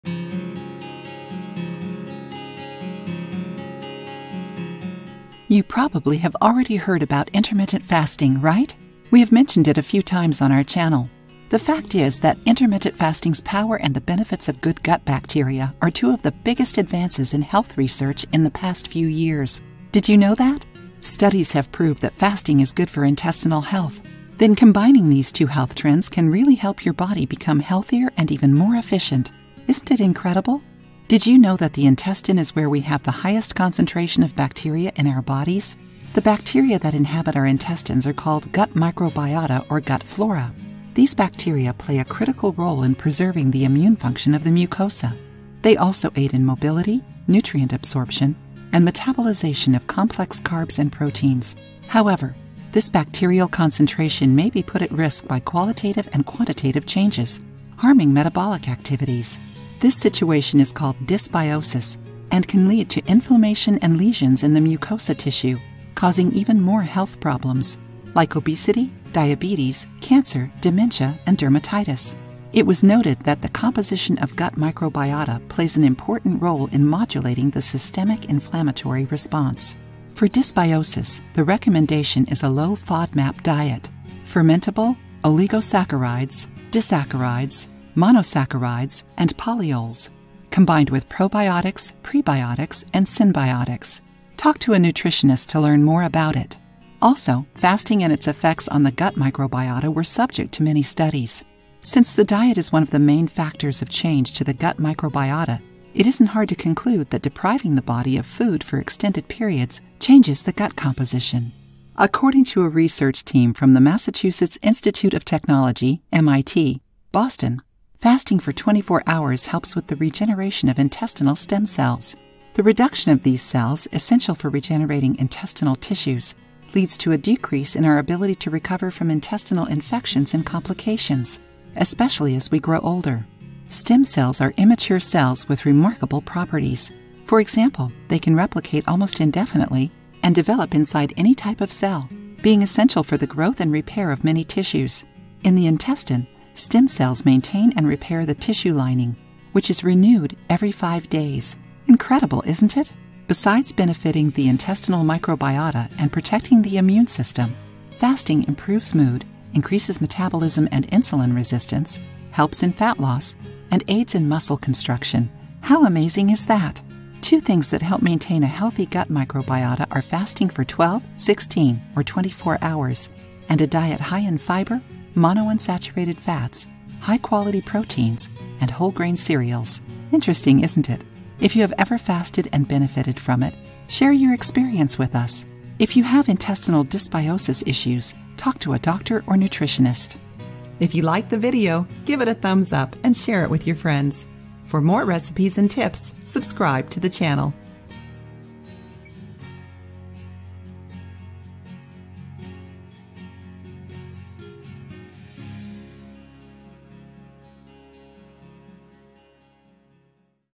How To Heal Your Gut With Intermittent Fasting
You probably have already heard about intermittent fasting, right? We have mentioned it a few times on our channel.

The fact is that intermittent fasting's power and the benefits of good gut bacteria are two of the biggest advances in health research in the past few years, did you know that?

Studies have proved that fasting is good for intestinal health.

Did you know that the intestine is where we have the highest concentration of bacteria in our bodies?

The bacteria that inhabit our intestines are called gut microbiota or gut flora.

These bacteria play a critical role in preserving the immune function of the mucosa. They also aid in mobility, nutrient absorption, and metabolization of complex carbs and proteins.

However, this bacterial concentration may be put at risk by qualitative and quantitative changes, harming metabolic activities.

This situation is called dysbiosis and can lead to inflammation and lesions in the mucosa tissue, causing even more health problems, like obesity, diabetes, cancer, dementia, and dermatitis.

It was noted that the composition of gut microbiota plays an important role in modulating the systemic inflammatory response.

Also, fasting and its effects on the gut microbiota were subjects to many studies.

Besides benefitting the intestinal microbiota and protecting the immune system, fasting improves mood, increases metabolism and insulin resistance, helps in fat loss, and aids in muscle construction, how amazing is that?

If you have ever fasted and benefitted from it, share your experience with us. If you have intestinal dysbiosis issues, talk to a doctor or nutritionist.